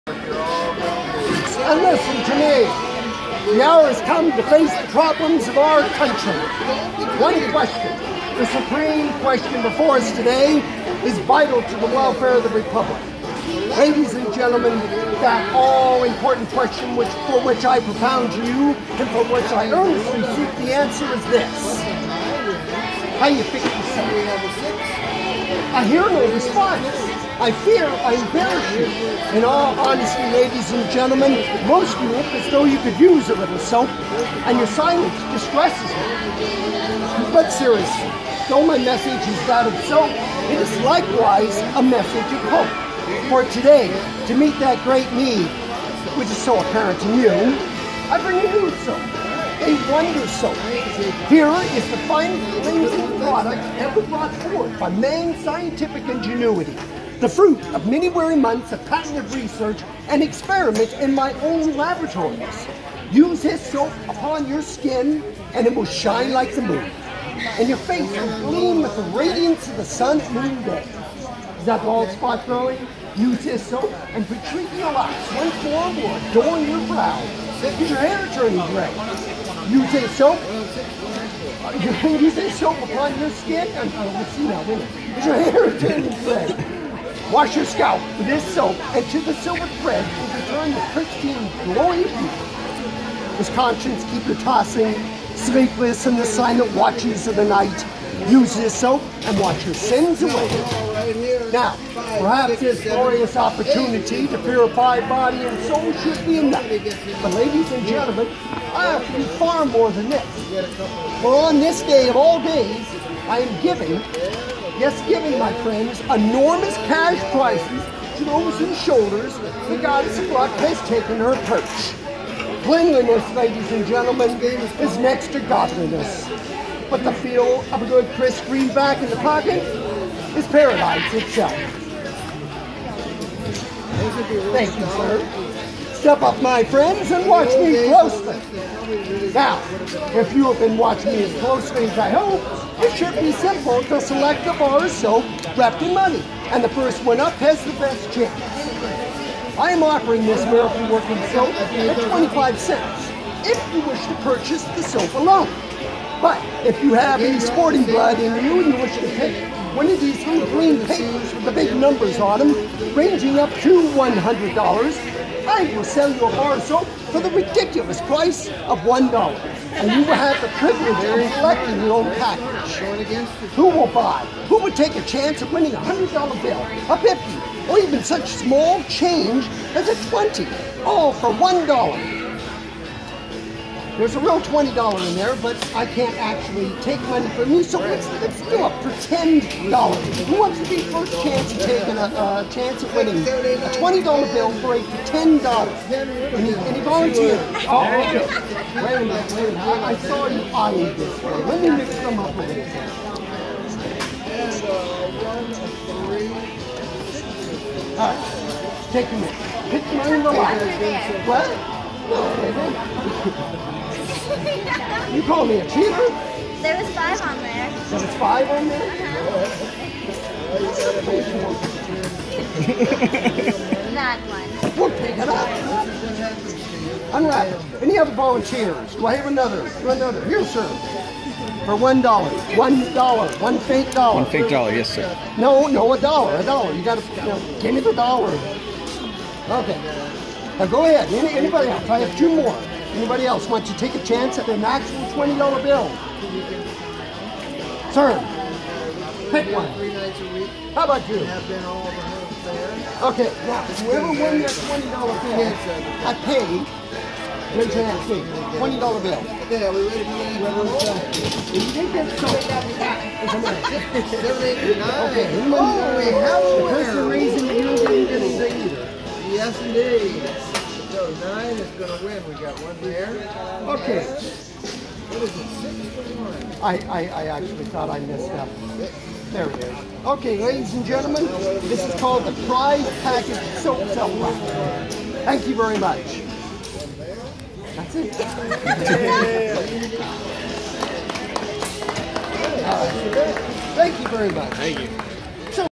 Jeff Smith, Soap Sell racket
Jeff performs the prize package soap sell racket.